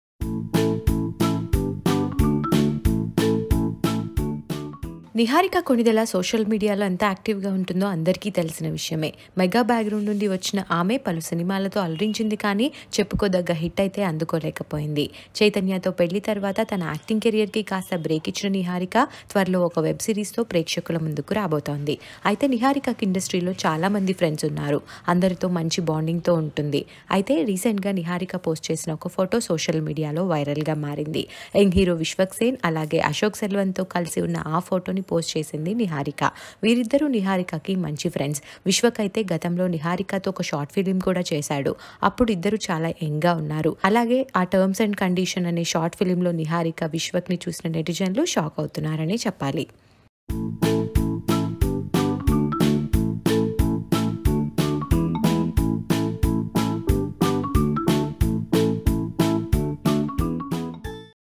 Niharika konidela with Vishwak sen latest pics goes viral | Gup Chup Masthi
Niharika konidela with Vishwak sen latest pics goes viral